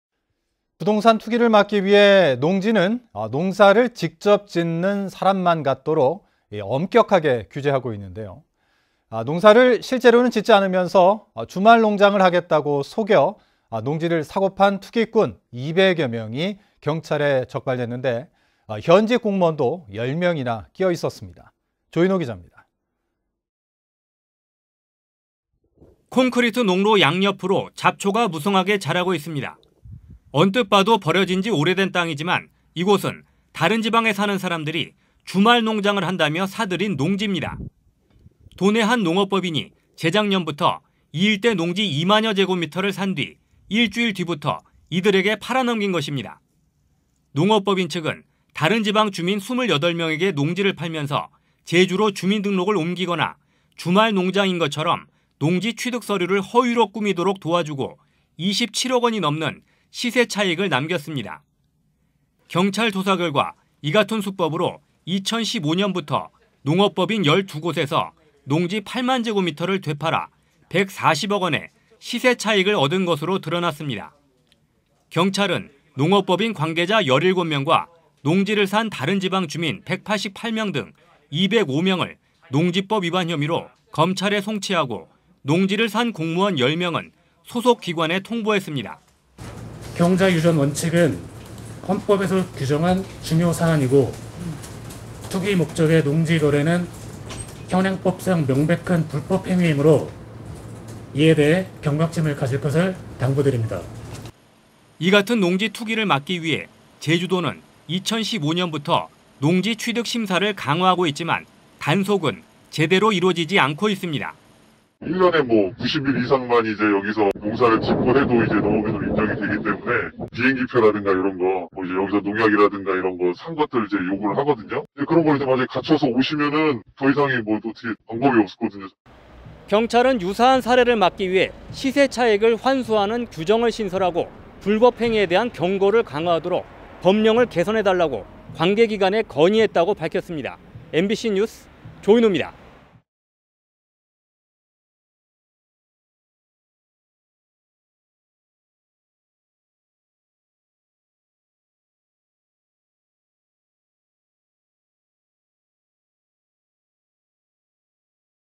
농지 투기꾼 200여 명 검거...공무원도 투기
콘크리트 농로 양 옆으로
잡초가 무성하게 자라고 있습니다.

 언뜻 봐도 버려진 지 오래된 땅이지만
이 곳은 다른 지방에 사는 사람들이
주말농장을 한다며 사들인 농지입니다.

 도내 한 농업법인이 재작년부터
이 일대 농지 2만여 제곱미터를 산 뒤
일주일 뒤부터 이들에게 팔아넘긴 것입니다.

 (c/g) 농업법인측은
다른 지방 주민 28명에게 농지를 팔면서
제주로 주민등록을 옮기거나
주말농장인 것처럼
농지취득서류를 허위로 꾸미도록 도와주고
27억원이 넘는 시세차익을 남겼습니다. (c/g)

 경찰 조사결과
이같은 수법으로 2천 15년부터
농업법인 12곳에서
농지 8만제곱미터를 되팔아
140억원의 시세차익을 얻은 것으로
드러났습니다.

 경찰은 농업법인 관계자 17명과
농지를 산 다른 지방 주민 188명 등 205명을
농지법 위반 혐의로 검찰에 송치하고
농지를 산 공무원 10명은
소속기관에 통보했습니다.

   김영운 / 제주지방경찰청 지능범죄수사대장          ◀ＩＮＴ▶
 "경자유전 원칙은 헌법에서 규정한 중요사안이고 투기 목적의 농지거래는 현행법상 명백한 불법행위이므로 이에 대해 경각심을 가질 것을 당부드립니다.'

 이같은 농지 투기를 막기 위해
제주도는 2천 15년부터
농지취득 심사를 강화하고 있지만
단속은 제대로 이뤄지지 않고 있습니다.

  농지 관리담당 공무원 ◀ＩＮＴ▶
 "1년에 90일 이상만 여기서 농사를 지어도
농업인으로 인정이 되기 때문에 비행기표라든지 농약을 산 것을 증거로 요구하는데
그런 것을 갖춰서 오면 더이상 방법이
없거든요."

"경찰은 유사한 사례를 막기 위해
시세차익을 환수하는 규정을 신설하고
불법행위에 대한 경고를 강화하도록
법령을 개선해달라고 관계기관에
건의했다고 밝혔습니다.